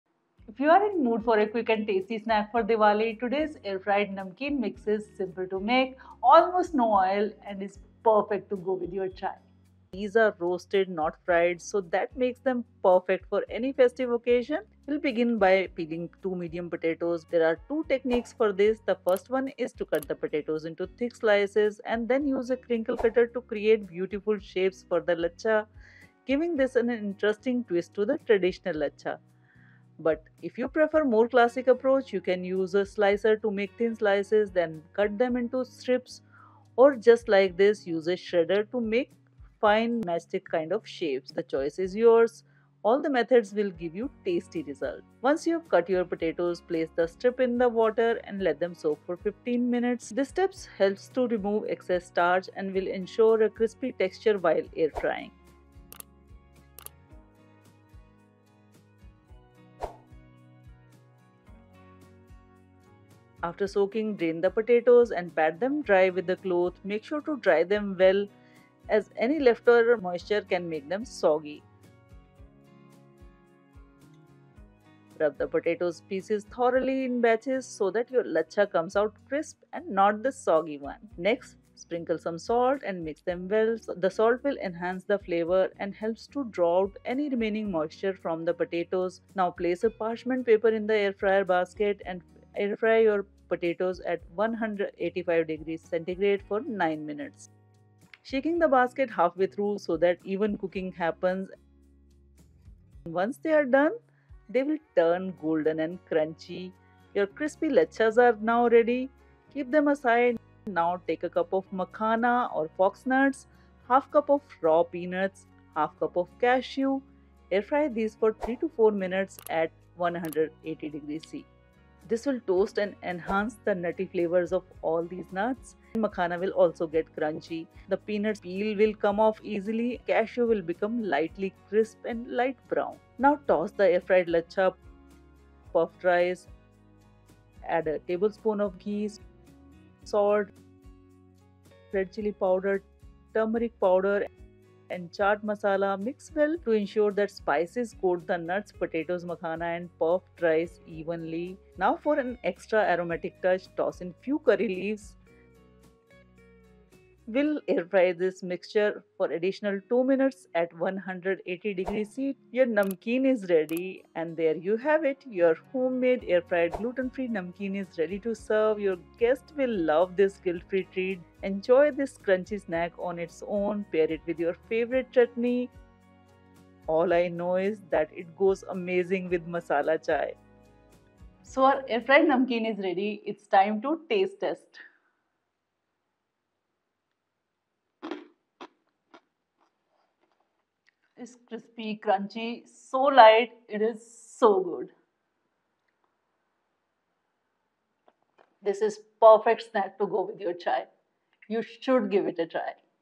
Roasted Namkeen Mixture For Diwali | Healthy Diwali Snack Recipe | Zero Oil Air Fryer Recipe
Roasted Namkeen Recipe in Air Fryer. It is healthy homemade Air fryer snack recipe you can make without oil.
So if you are looking for vegetarian gluten-free snack that is made with almost zero oil, this quick and easy snack recipe you can make in air fryer is crispy treat without the need for deep frying.

Perfect for those craving a healthy alternative to traditional fried snacks, this roasted Chiwda mix is packed with the goodness of makhana or foxnut, peanuts, cashews, and a hint of spices.

Whether it's for tea-time or an on-the-go snack, this air-fried namkeen will satisfy your cravings guilt-free!

Ingredients:
2 potatoes (for laccha)
1 cup makhana (fox nuts)
¼ cup peanuts
¼ cup cashews
½ teaspoon red chili powder
1 teaspoon chaat masala
1 teaspoon cumin powder
1 teaspoon ghee
Salt to taste